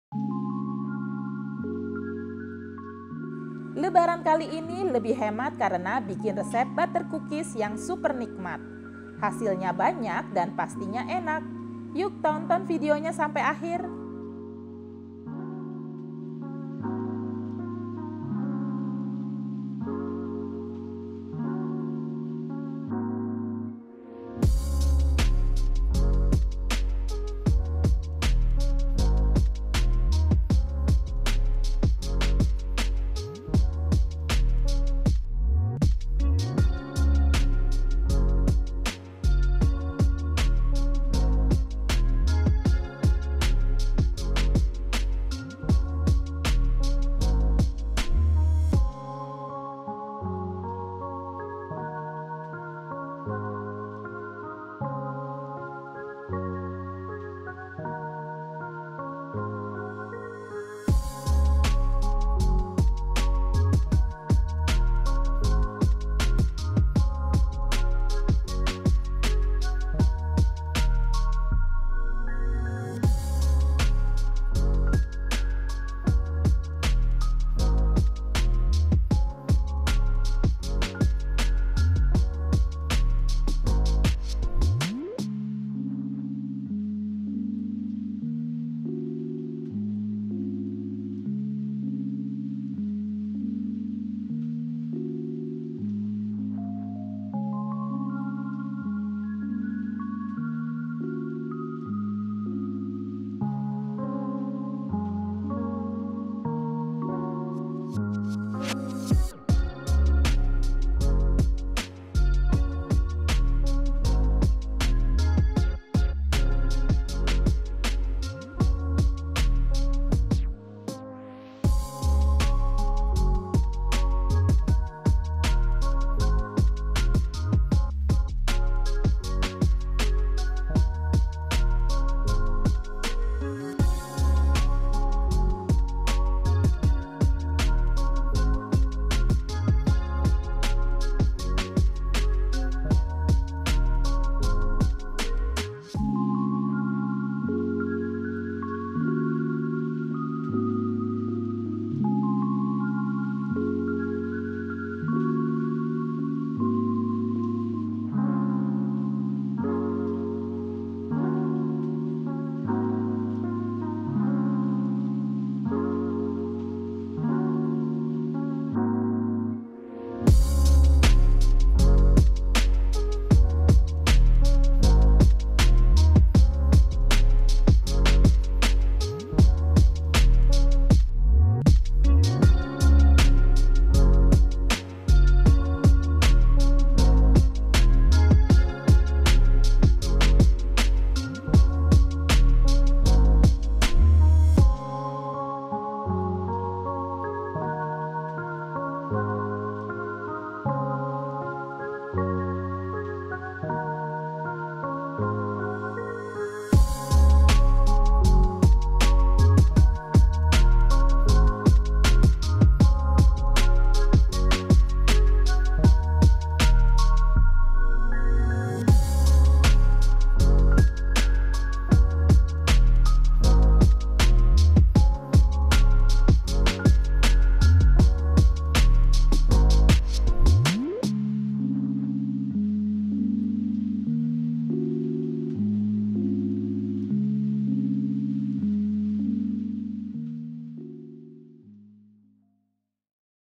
Resep Lotus Butter Cookies, Resep Kue Kering Terbaru 2021 Buat Lebaran
Mau cari resep kue kering terbaru 2021? Cek resep Lotus Butter Cookies yang enak ini untuk Lebaran.

00:00- intro
00:19- bahan-bahan lotus butter cookies
00:41- cara membuat lotus butter cookies
02:10- cara menyemprot adonan kue kering
02:24- cara memberi topping
02:58- cara mengoven Lotus Butter Cookies
03:44- hasil jadi lotus butter cookies
03:53- resep lotus butter cookies

Produser: Intan Yusan Septiani
Penulis Naskah: Qorin & Indri
Peraga: Qorin, Resi & Indri
Fotografer: Bambang Tetuko
Videografer: Bambang Tetuko
Video Editor: Bambang Tetuko